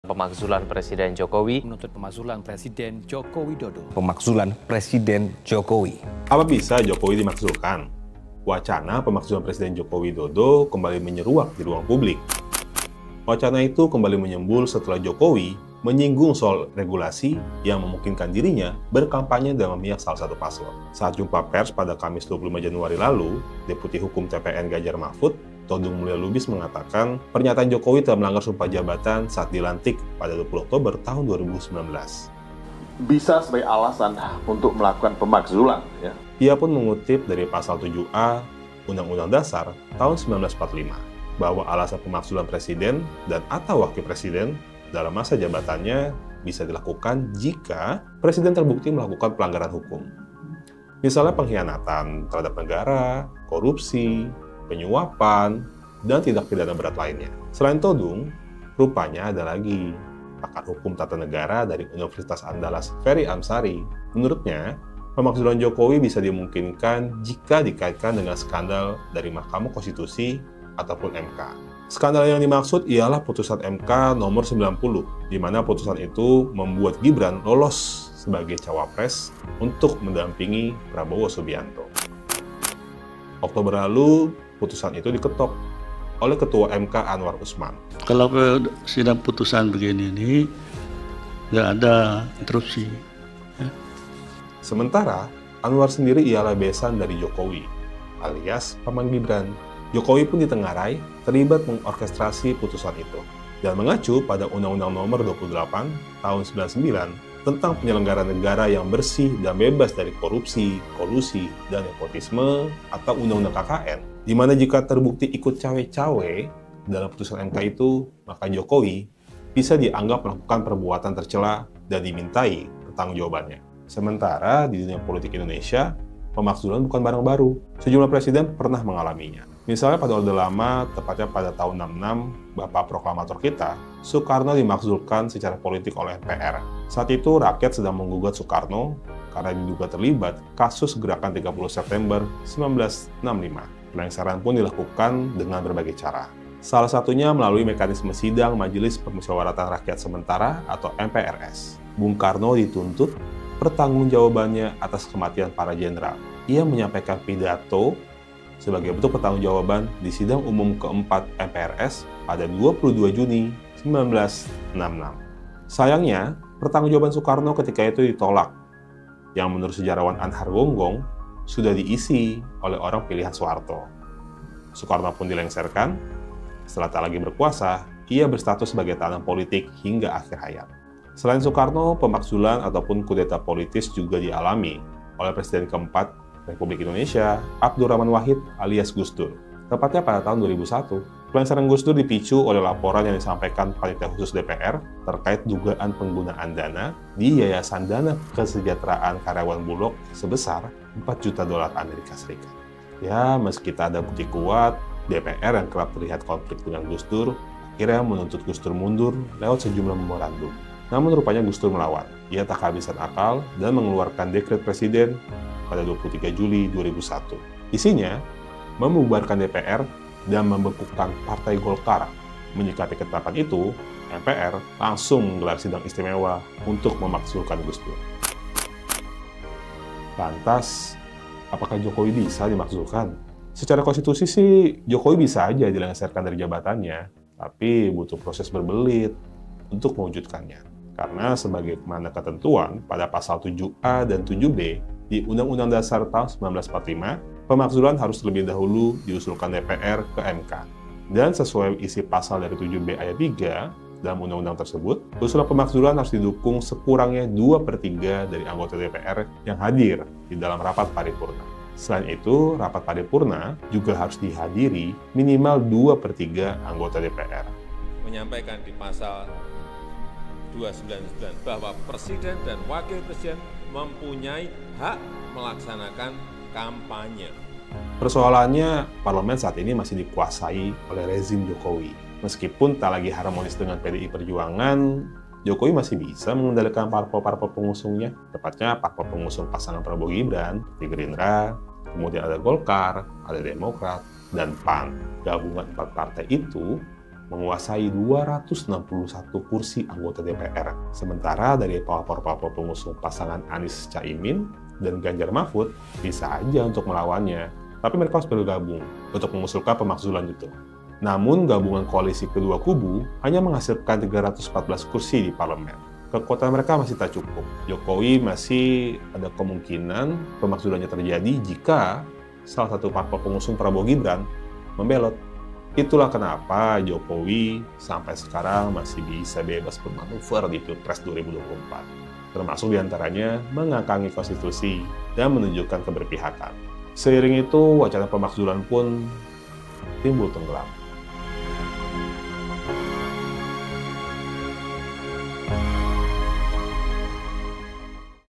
Apa bisa Jokowi dimakzulkan?
Sejak beberapa waktu lalu, beredar wacana memakzulkan Presiden Jokowi. Presiden Jokowi diduga memiliki keterlibatan dalam Pemilu 2024.

Presiden Joko Widodo juga diduga terindikasi melakukan kecurangan pemilu dengan memanfaatkan kekuasaannya.

Tetapi apa benar Presiden Jokowi bisa dimakzulkan karena alasan tersebut? Teman Alinea bisa menemukan jawabannya dengan menonton sampai habis video ini.